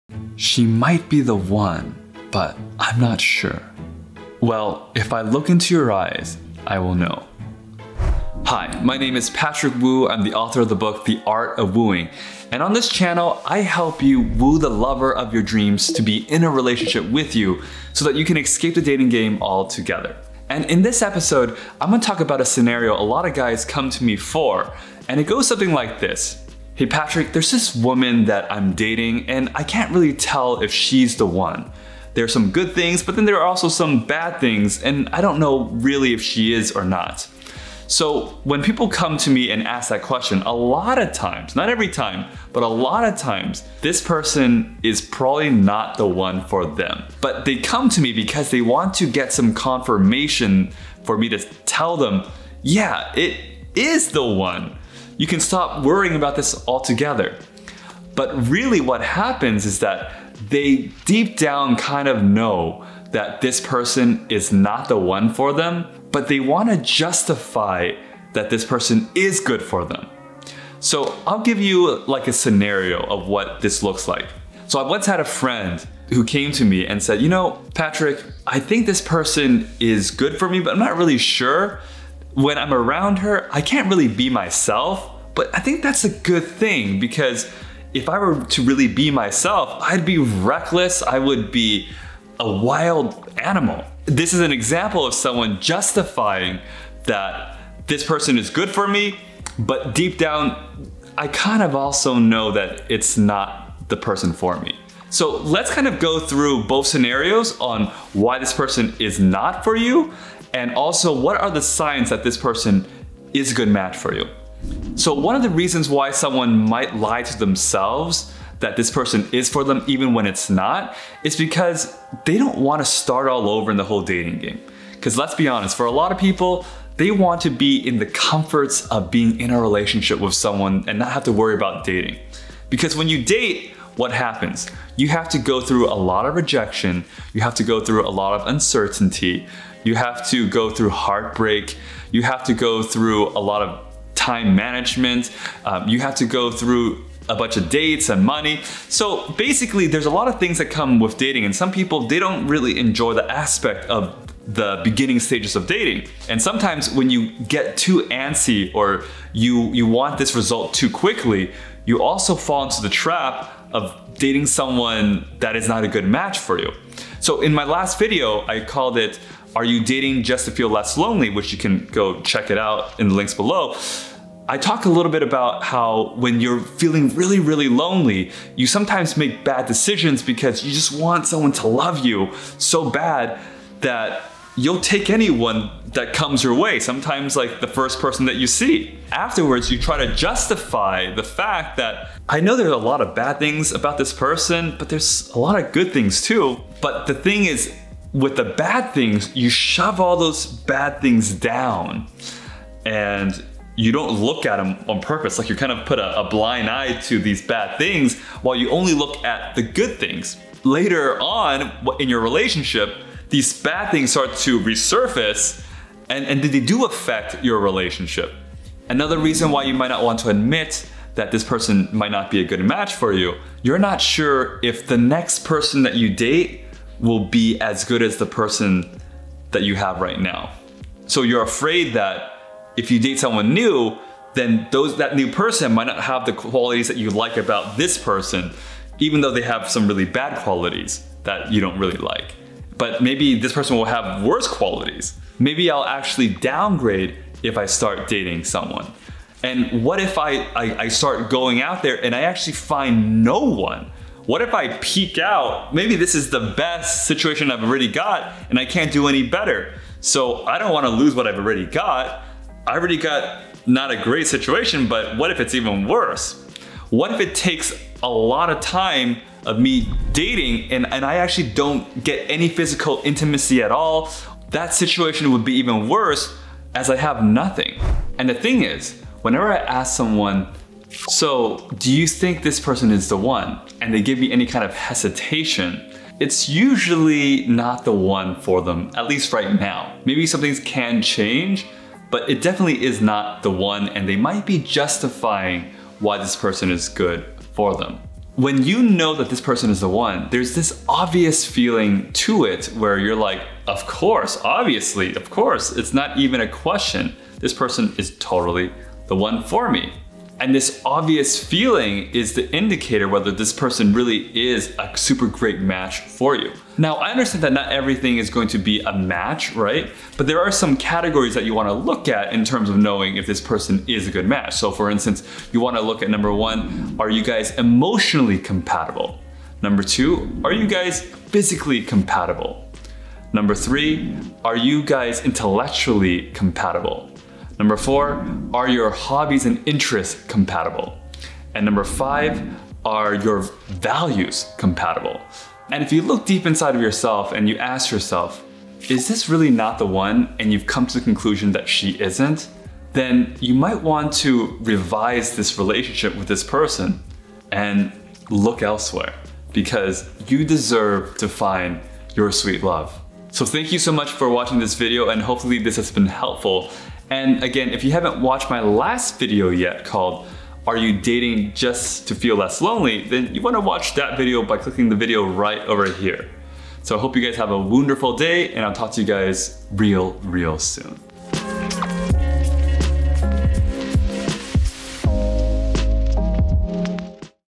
The Test To Know If She Is “The One"
Often times, I get asked the question, "Is she the one for me"? I'm not sure. Well I'm not in your shoes so I don't know the answer. However, from an outside point of view, here are some key things to look at to determine if she really is the sweet love that you seek.

To Read Or Listen To My Book: The Art Of Wooing : 9 Ways Of A Super Charmer